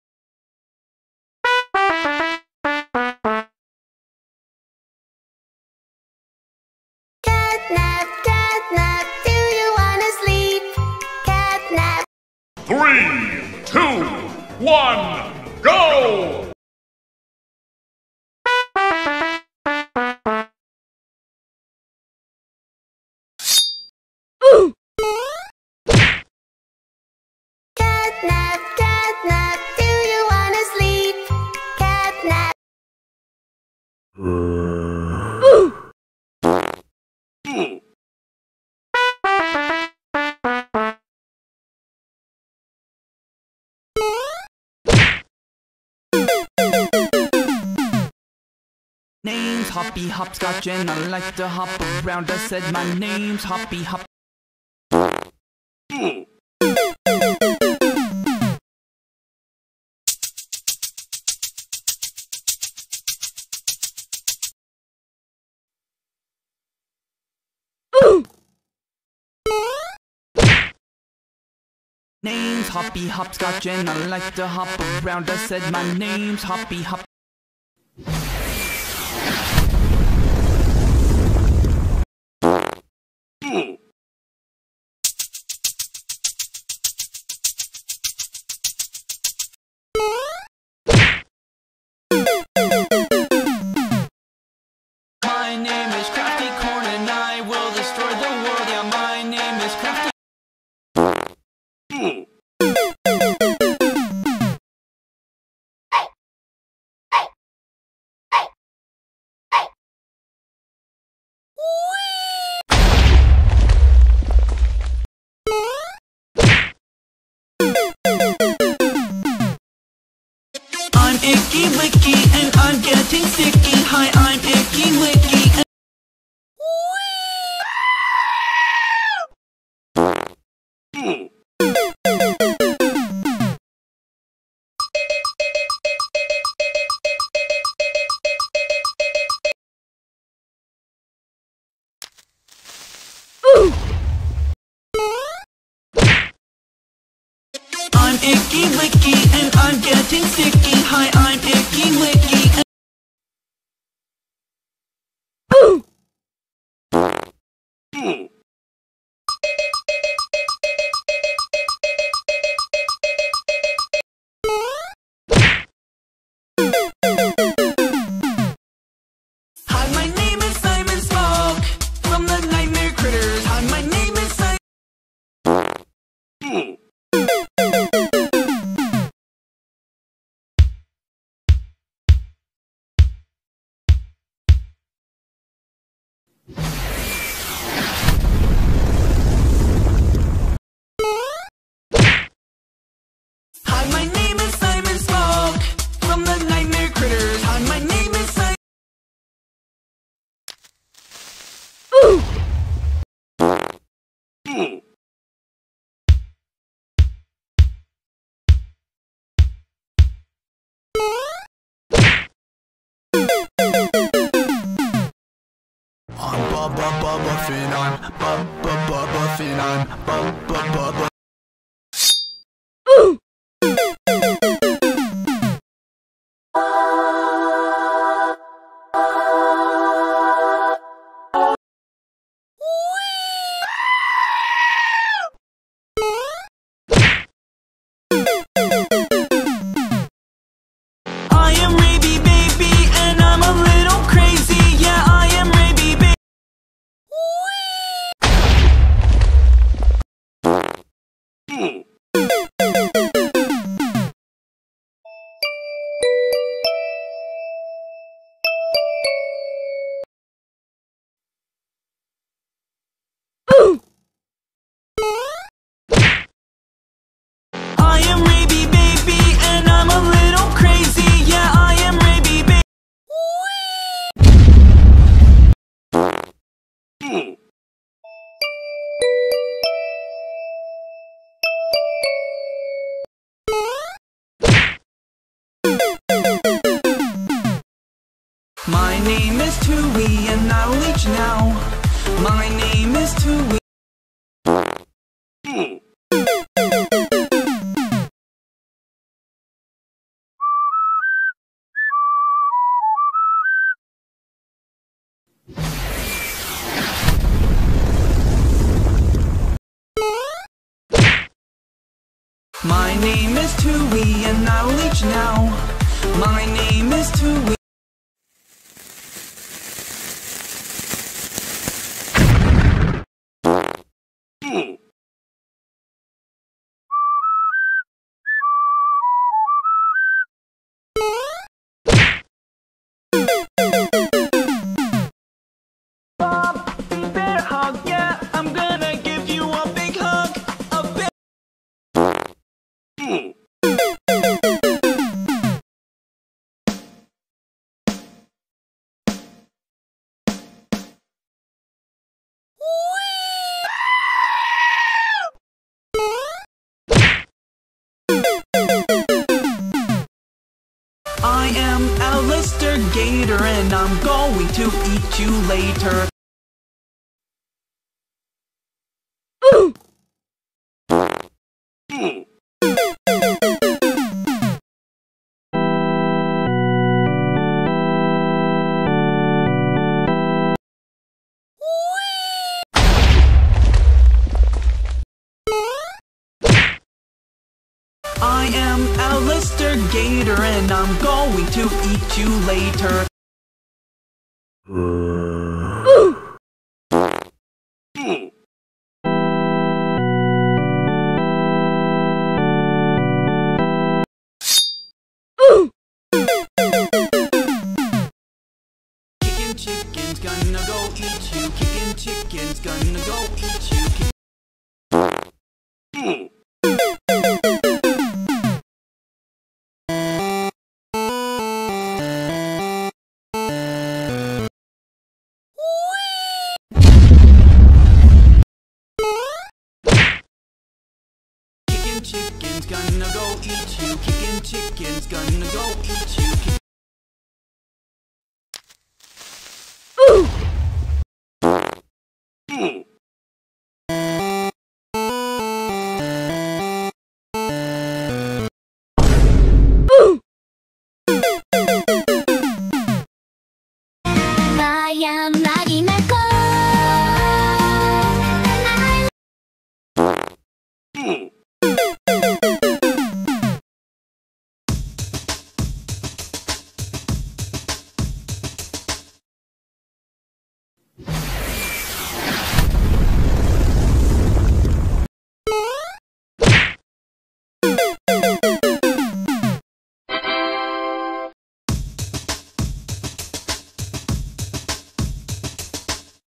All Sprunki Characters vs All Poppy Playtime Characters! Meme battle
Hi guys! In this video you will see the battle of All Sprunki Characters vs All Poppy Playtime Characters! Be sure to watch this video to the end and write who will be winner! Thanks)